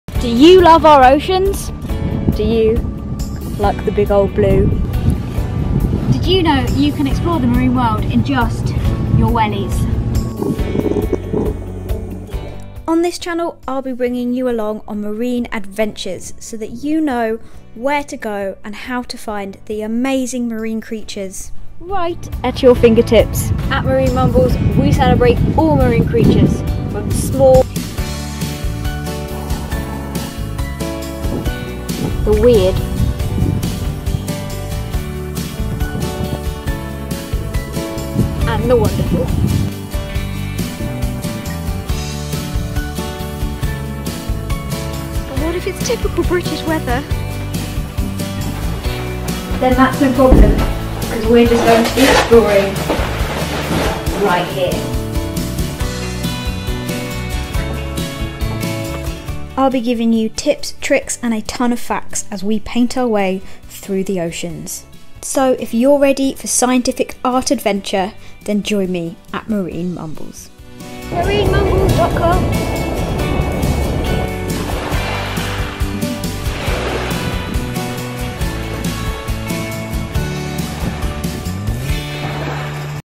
CHANNEL TRAILER: Welcome to MarineMumbles! (Marine biology + Scientific Art)
This is a place to let your love of oceans run wild! Here were be exploring the seas, where I will show you the wonders of the marine world and how to find them for yourself! If I can’t find them, then we’ll be painting them as I tell you tips tricks and a ton of facts as we paint our way through the ocean! Welcome to MarineMumbles!